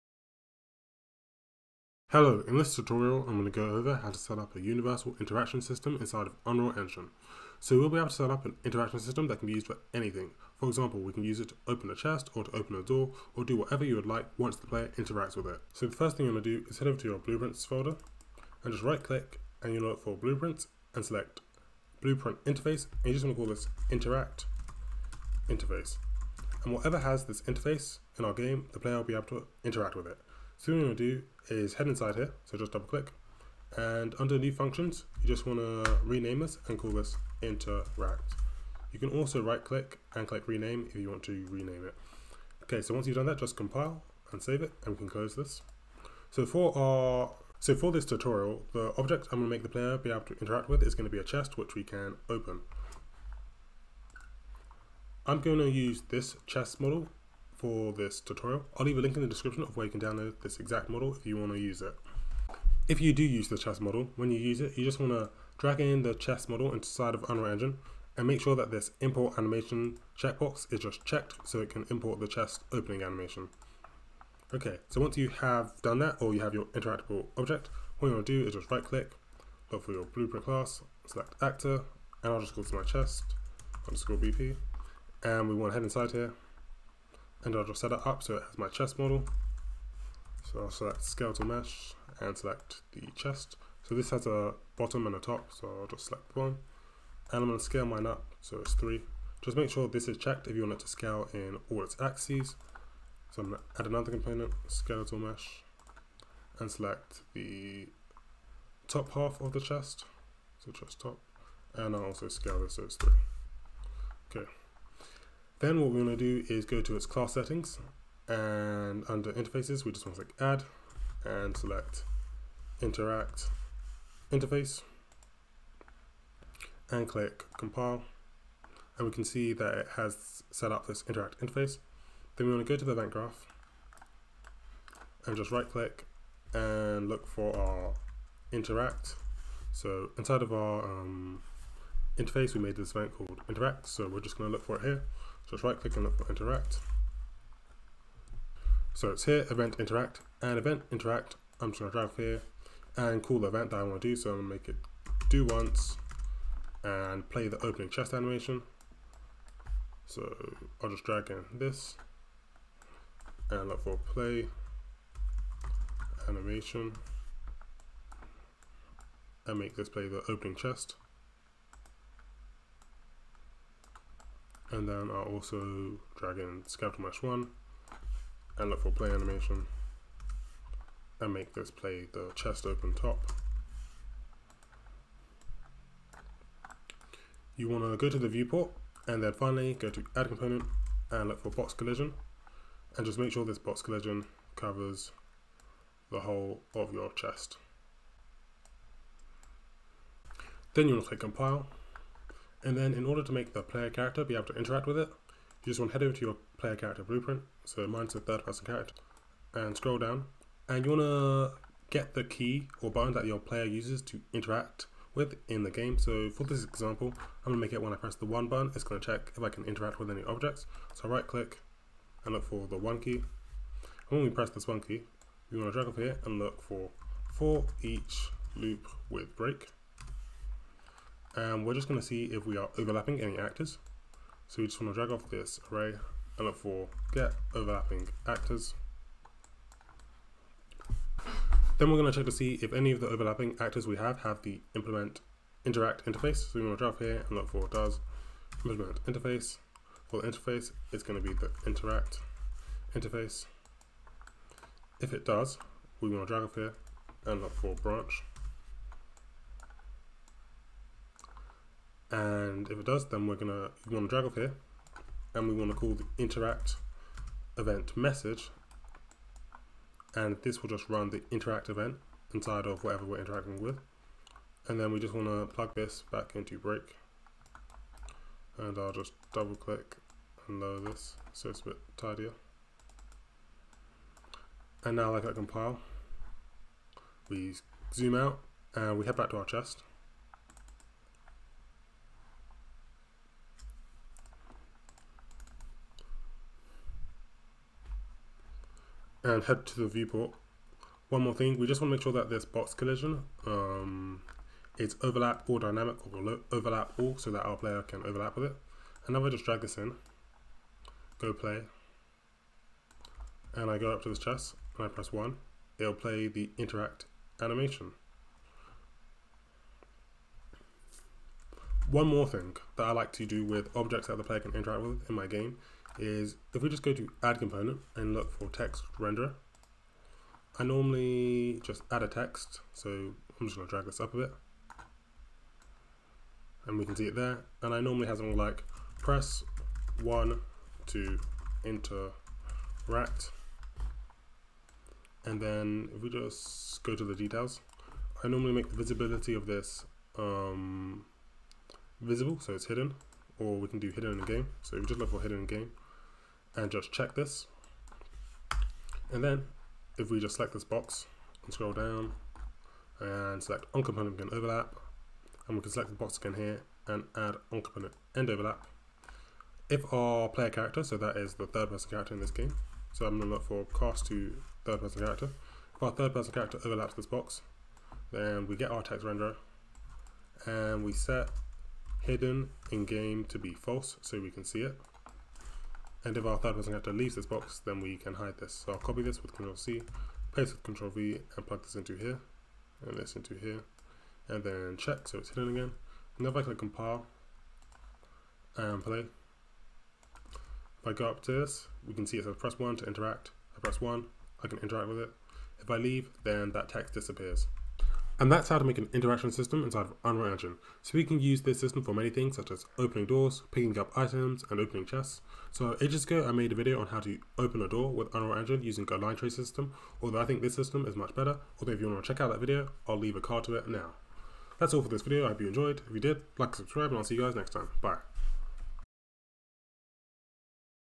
Unreal Engine Interaction System Tutorial
In this tutorial I go cover how to make an interaction system in Unreal Engine